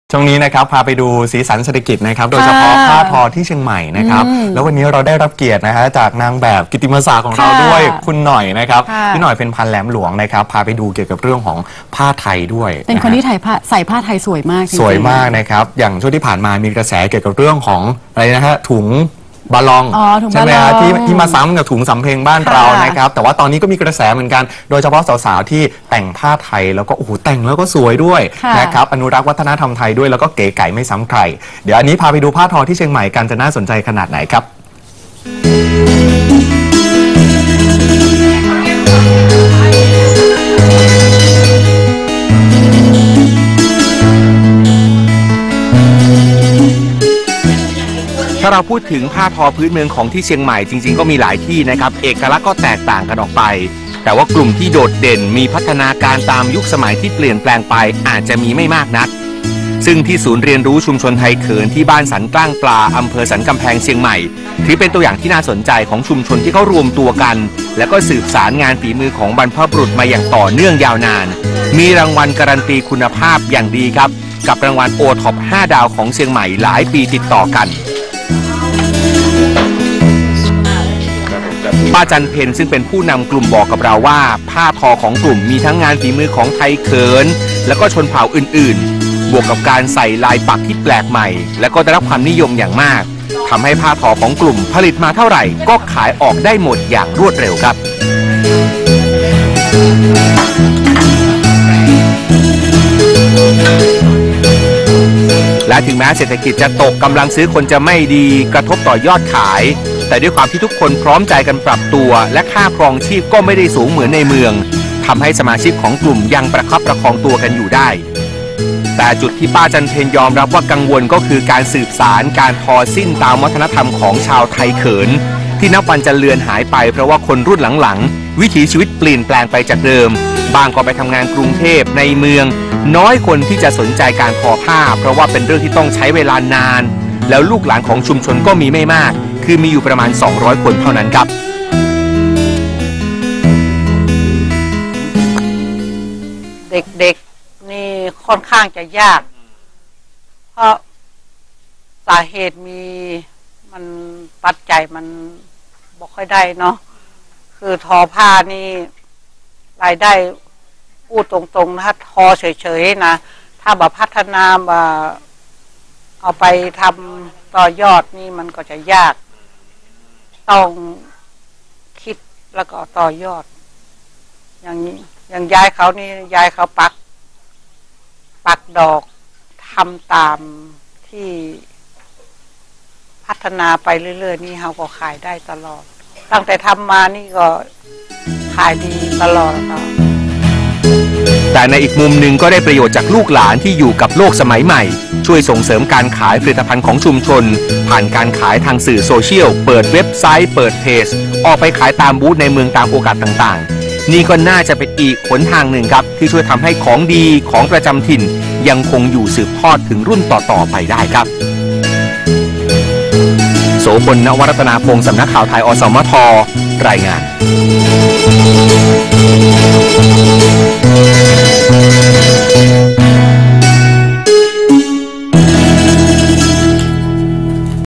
สีสันเศรษฐกิจ : ผ้าทอ ศูนย์เรียนรู้ชุมชนไทยเขิน เชียงใหม่ | สำนักข่าวไทย อสมท
“สีสันเศรษฐกิจ” วันนี้ พาไปที่ จ.เชียงใหม่ ไปดูงานผ้าทอพื้นเมืองที่ใช้ความคิดสร้างสรรค์ บวกกับภูมิปัญญาท้องถิ่น ผลิตผ้าทอที่ได้รางวัลโอท็อป 5 ดาว มาหลายปีติดต่อกัน